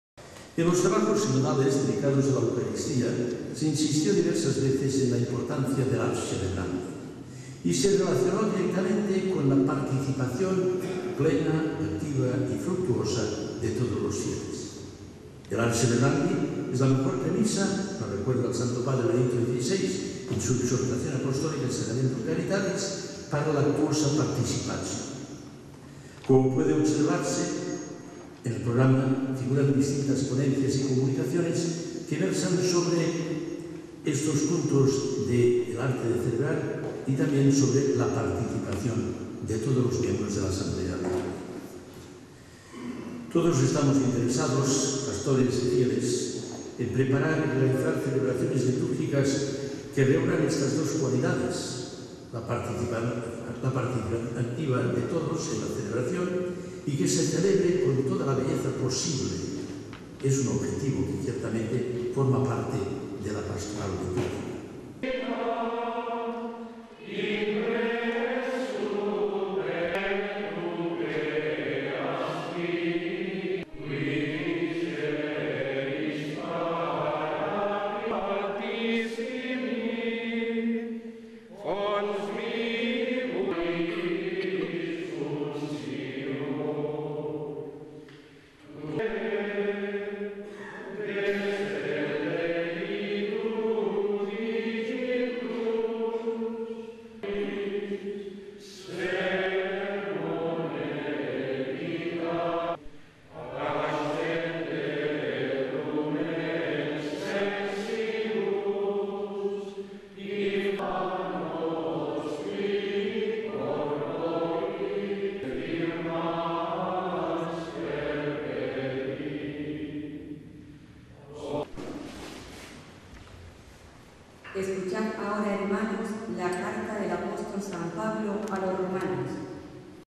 Sistach pide participación en las celebraciones
El Cardenal-Arzobispo de Barcelona, Lluís Martínez Sistach, ha inaugurado esta tarde las Jornadas Nacionales de Liturgia 2011 en Barcelona donde ha destacado la necesidad de la participación activa en las celebraciones de liturgia. Las jornadas han contado con un centenar de asistentes.